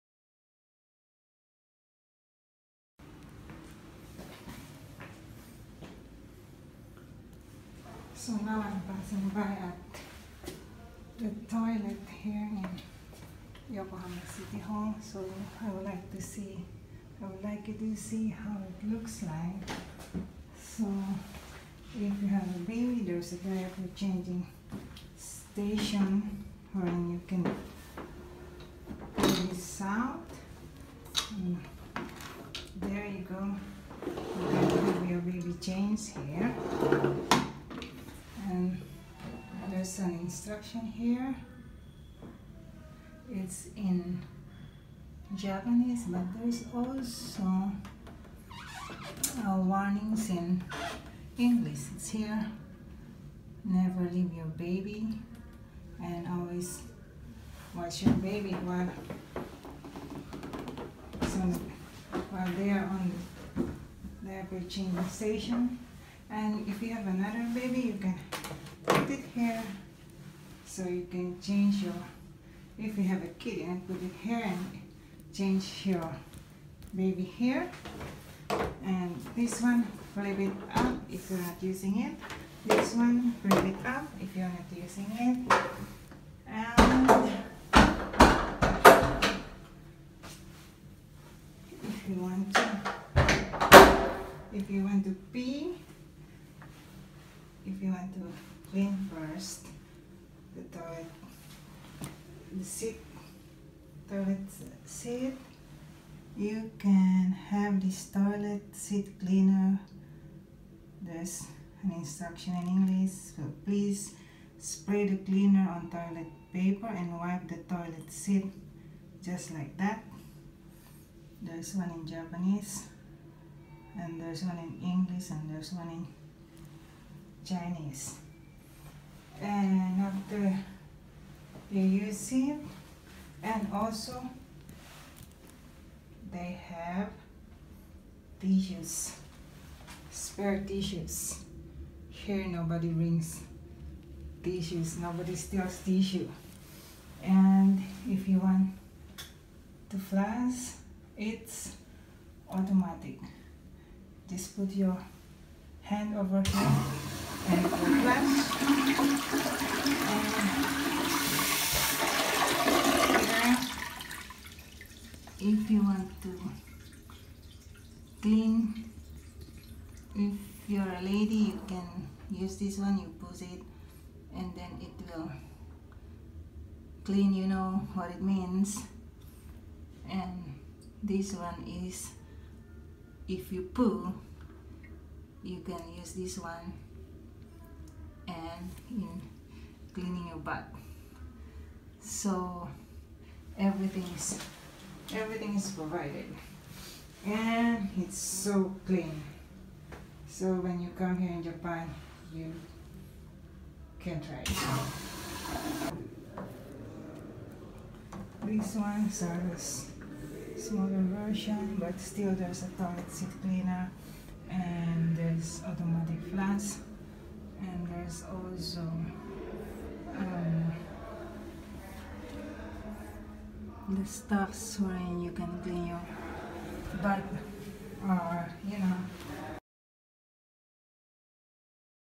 A Glimpse Of Japan’s Public Toilet!
Japan's Public Toilet are basically good, maintaining cleanliness and are free.